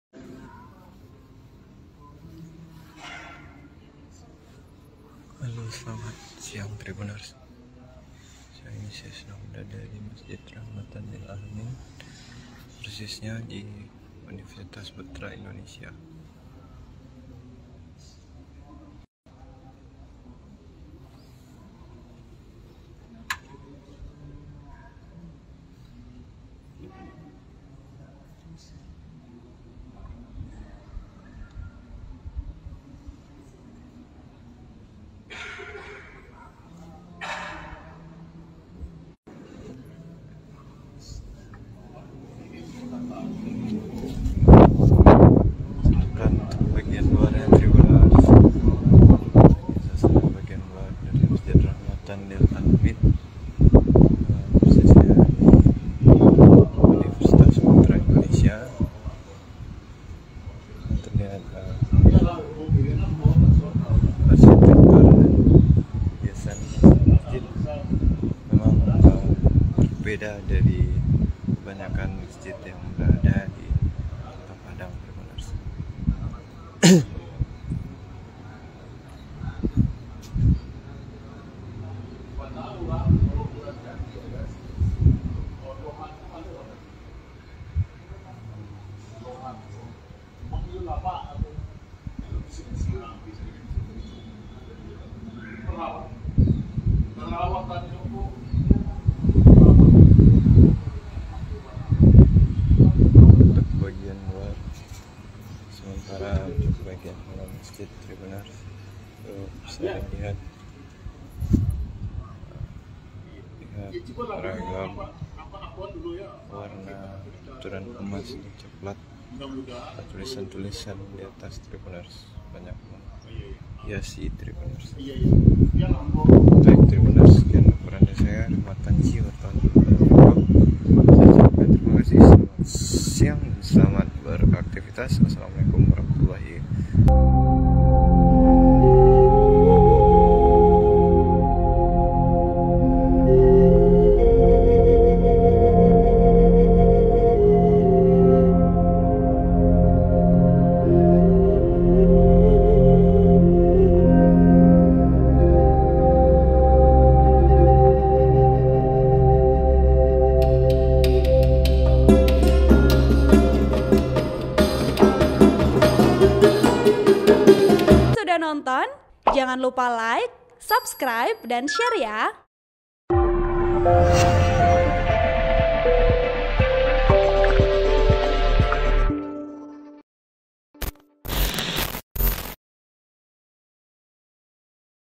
Mengenal Masjid Rahmatan Lil 'Alamin yang Berada di Kawasan UPI, Gaya Arsitektur Timur Tengah
TRIBUN-VIDEO.COM - Bila berkendara di Jalan Raya Lubuk Begalung di kawasan Universitas Putra Indonesia (UPI) Kota Padang akan terlihat Masjid Rahmatan Lil 'Alamin dengan warna dominan emas yang menarik perhatian.

Dengan khubah besar berwarna emas itu membuat Masjid Rahmatan Lil 'Alamin memiliki ciri khas sendiri.

Campuran warna emas dan putih membuat masjid ini terkesan megah dengan ukiran-ukirannya.(*)